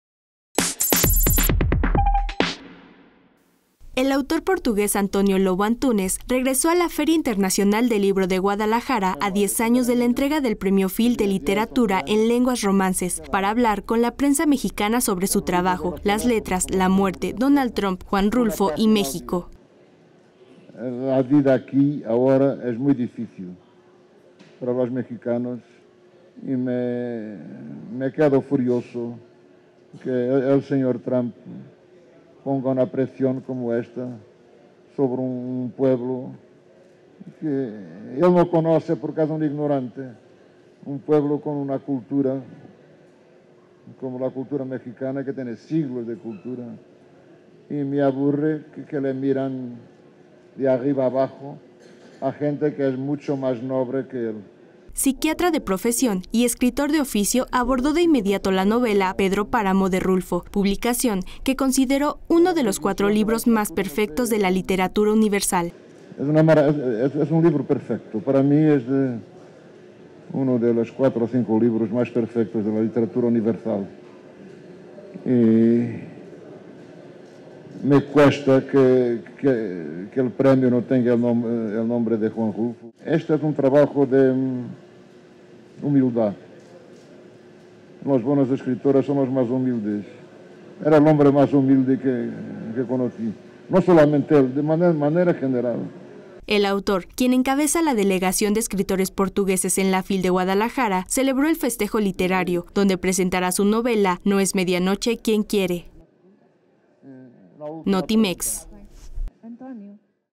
Rulfo, el hombre más humilde que conocí: Lobo Antunes
El autor portugués Antonio Lobo Antunes regresó a la Feria Internacional del Libro de Guadalajara (FIL)  para hablar sobre su trabajo literario, las letras, la muerte, Donald Trump, Juan Rulfo y México.